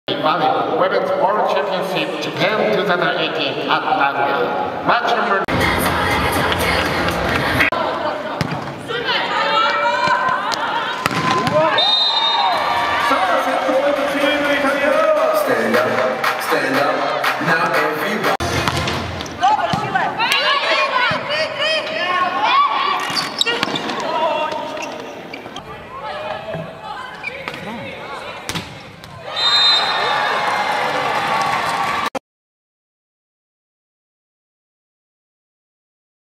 Mondiali Femminili 2018: LaNazionale battuta dalle vicecampionesse olimpiche della Serbia 1-3
Nel Campionato Mondiale femminile 2018 è arrivata la prima sconfitta per la nazionale azzurra, battute dalle vicecampionesse olimpiche della Serbia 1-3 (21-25, 19-25, 25-23, 23-25). Il ko non pregiudica il cammino dell’Italia, già qualificata alle semifinali di Yokohama (19 ottobre), così come le serbe.